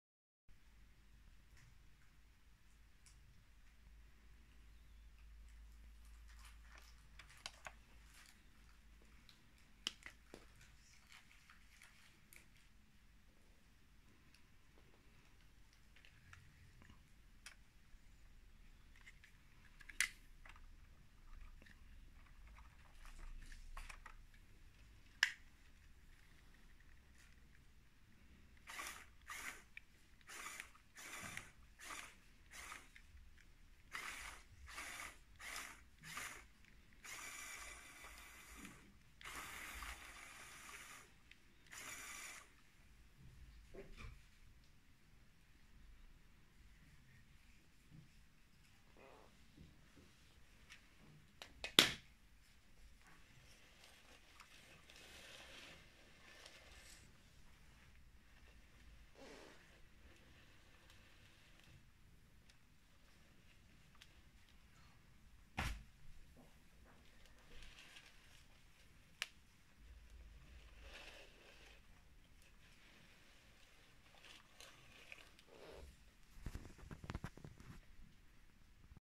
[WIP] Small APM for LEGO Rock Concert (5)
Track is gaining shape (not the proper colors yet).